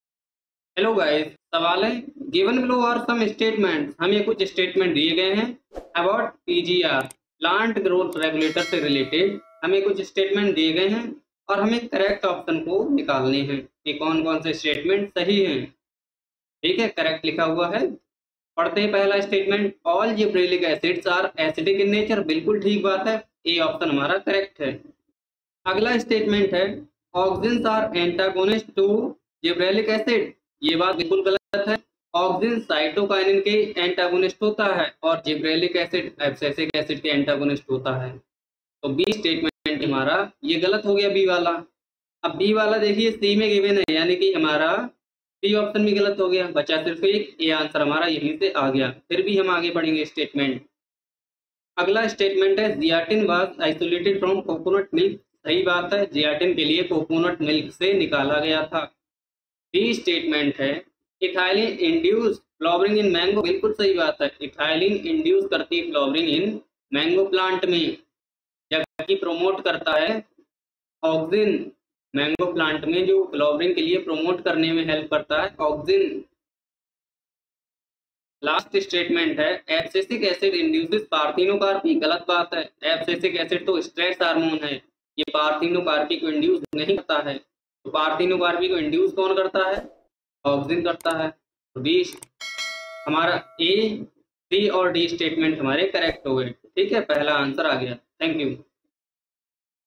Given below are some statements about plant growth regulators. A. All GAs are acidic in nature.
Given below are some statements about plant growth regulators. A. All GAs are acidic in nature. B. Auxins are antagonists to GAs. C. Zeatin was isolated from coconut milk. D. Ethylene induces flowering in Mango. E. Abscisic acid induces parthenocarpy. Choose the correct set of statements from the options given below: (1) A, C, D (2) B, E (3) A, B, C (4) B, D, E